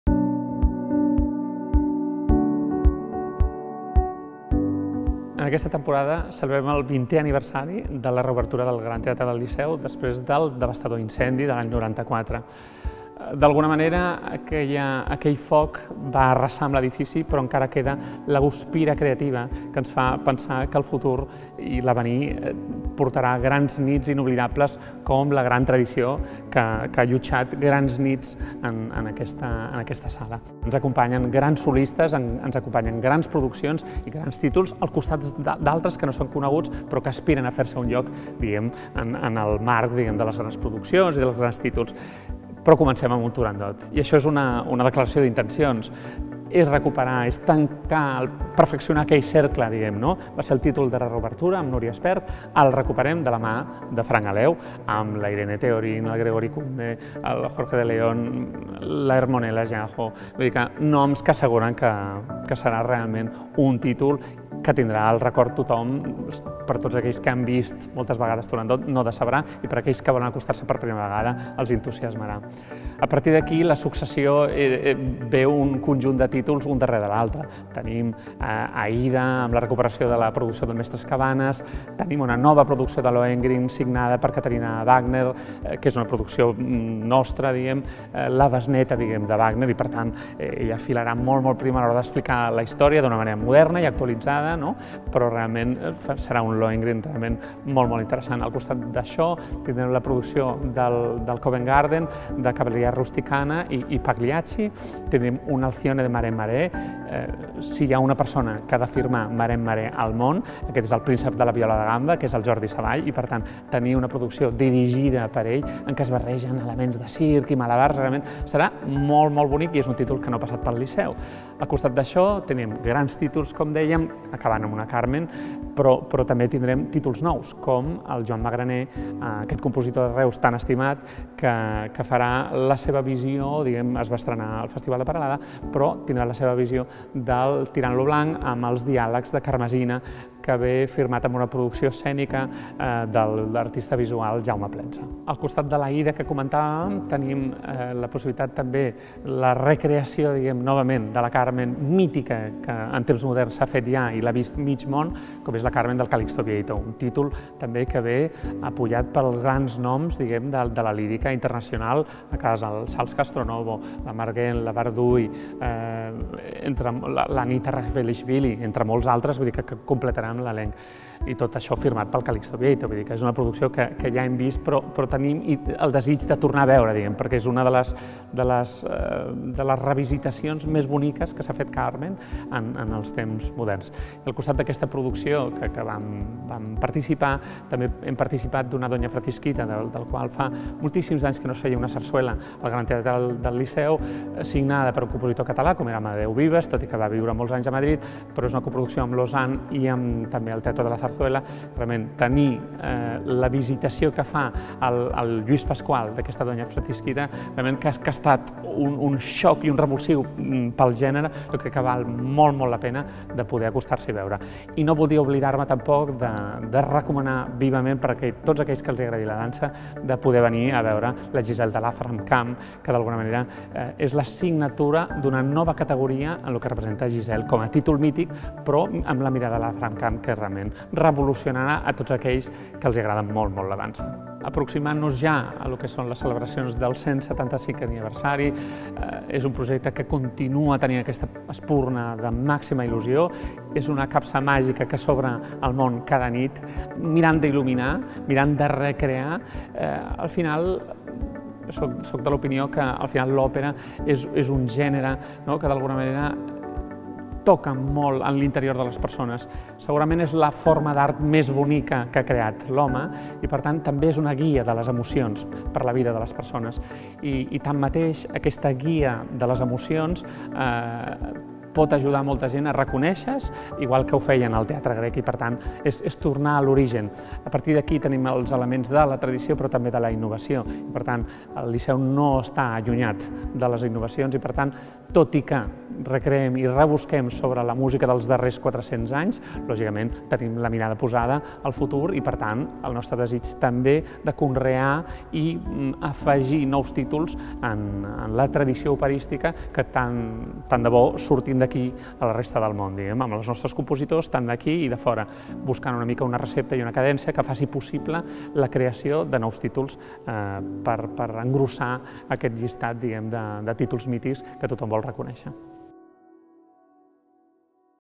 Víctor García de Gomar, director artístic del Liceu
El director artístic del Liceu, Víctor García de Gomar, exposa les claus del vintè aniversari del Liceu.